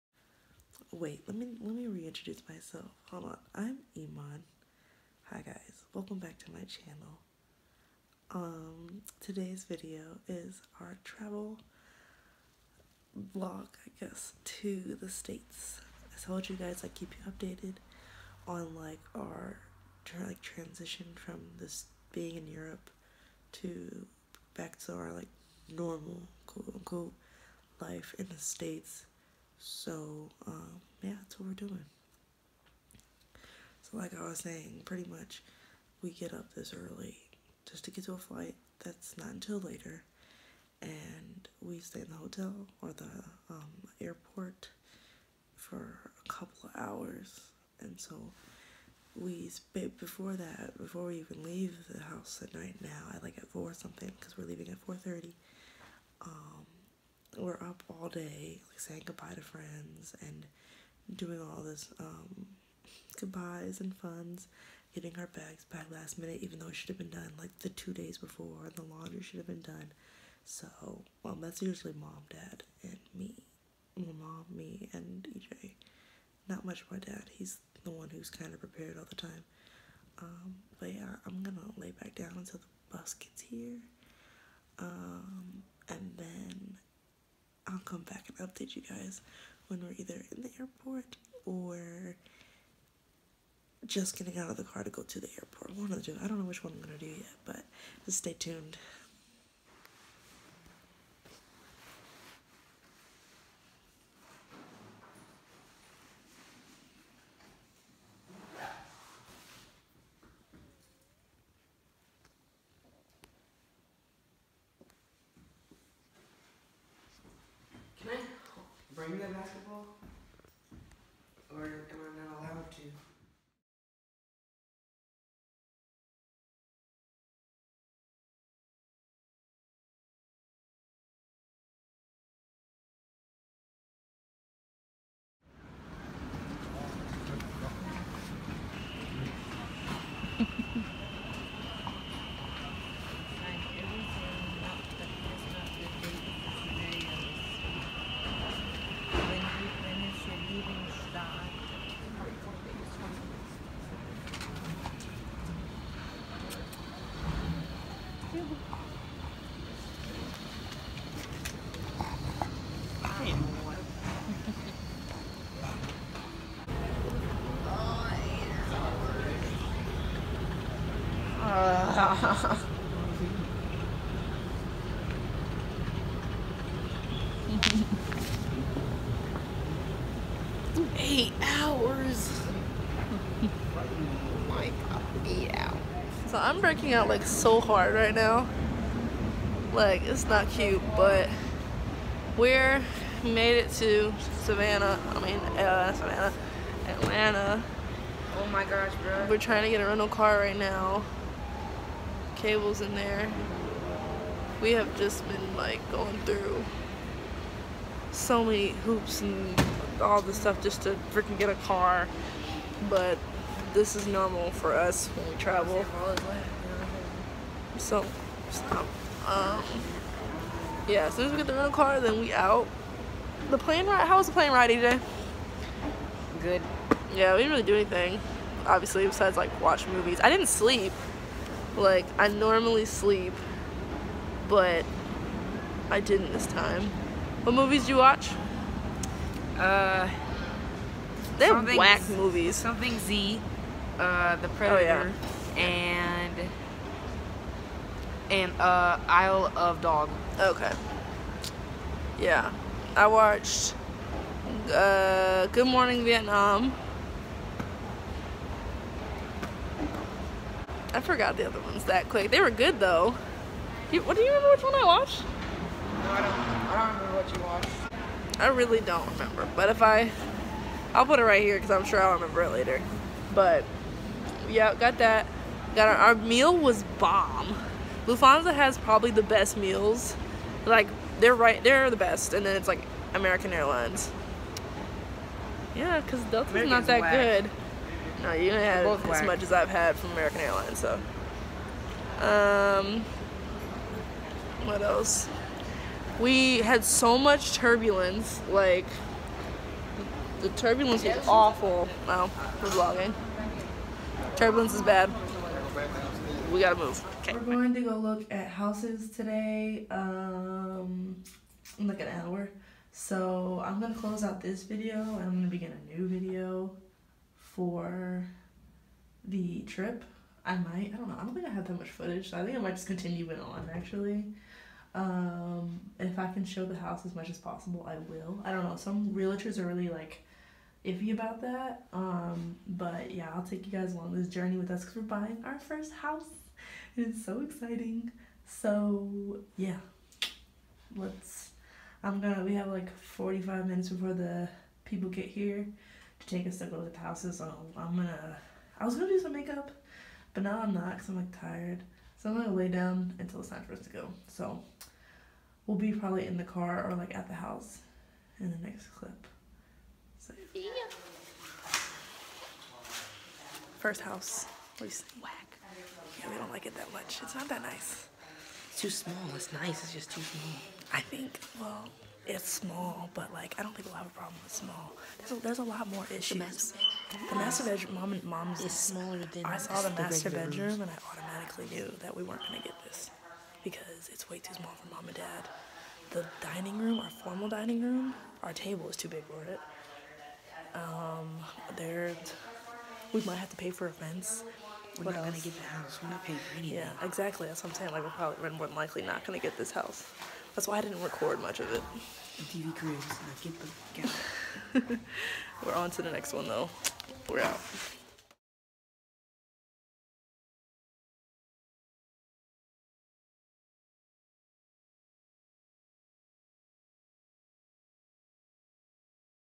Germany To Alabama 🥳 Here We Go!
We’re back in the states!!’ Yay!! Okay guys don’t hate me I edited this video off of my phone so give me so slack! More videos to come. okay love you bye.